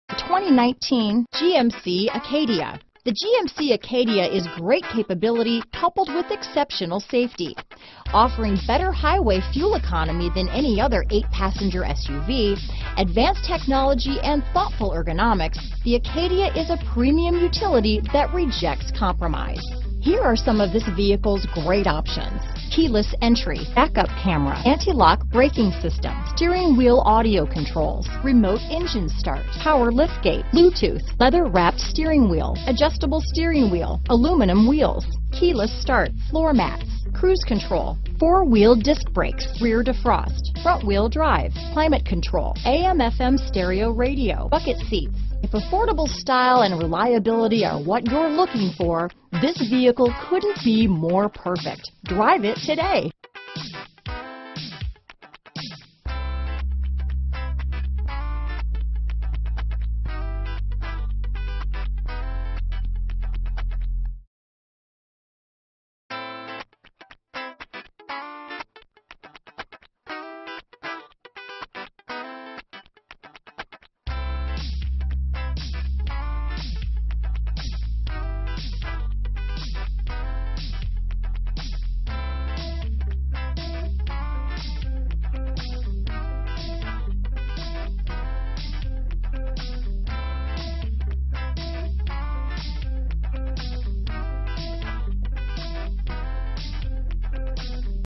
2019 GMC Acadia Jacksonville, Greenville, New Port, Morehead City, Wilmington, NC GN5146
Quicksilver Metallic New 2019 GMC Acadia available in New Bern, North Carolina at Trent Cadillac Buick GMC. Servicing the Jacksonville, Greenville, New Port, Morehead City, Wilmington, NC area.